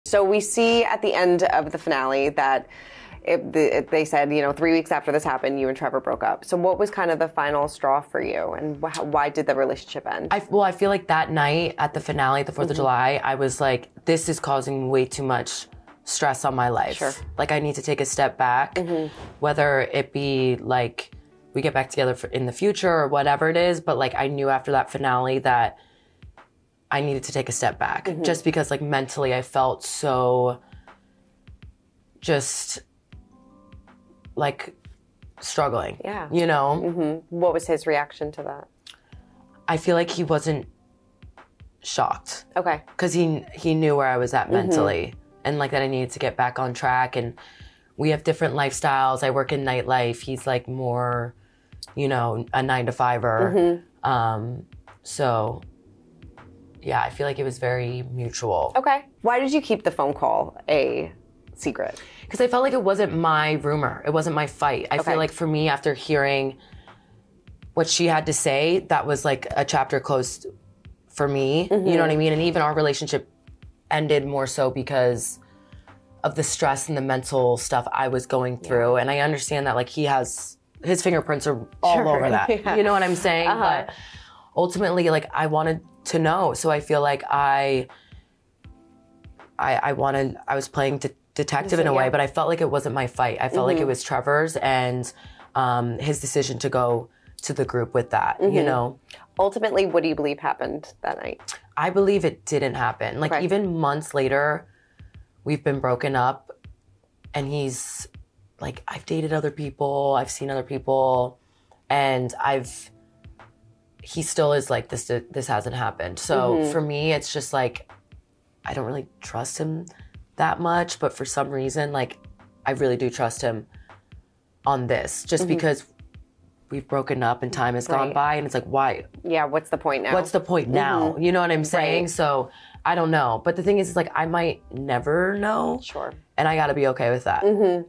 Southern Hospitality Maddi Reese Reveals Why She Doesn't Believe Trevor Cheated In Season 2
VPR Tom Sandoval Called Out By Jax Taylor & Paige DeSorbo Reacts To Craig Conover Hookup Claims